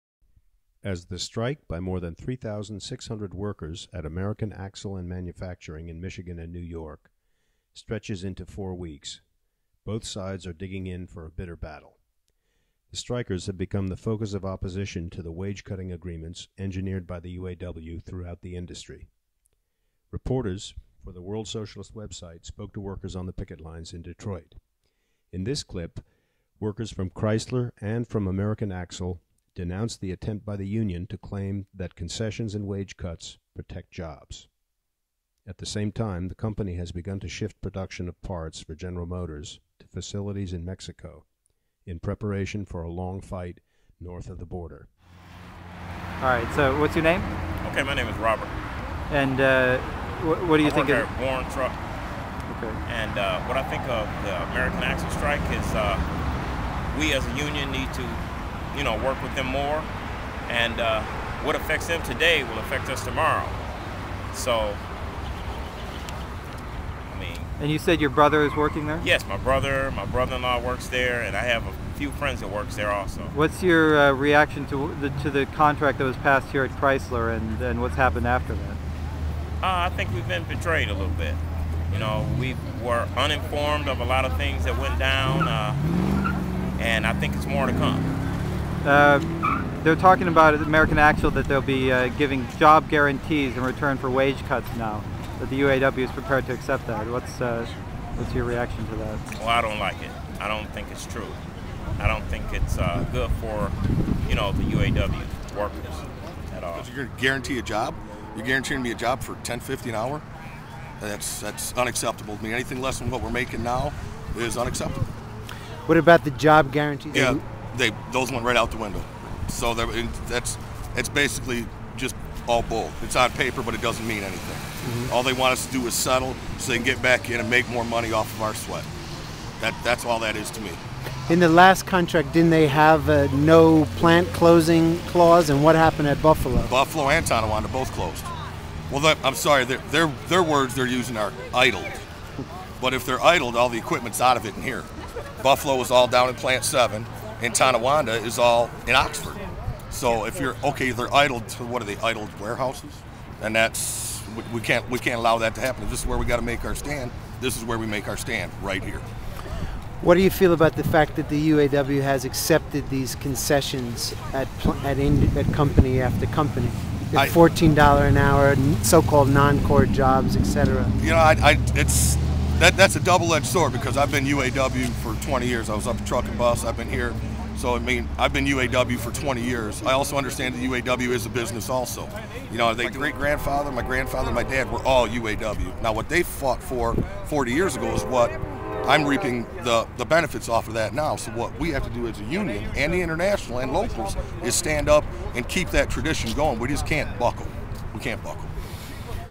American Axle and Chrysler workers in Detroit speak out
The World Socialist Web Site spoke with striking American Axle & Manufacturing (AAM) workers, and Chrysler workers supporting their struggle, as the strike by 3,650 AAM workers at plants in Michigan and western New York enters in fourth week.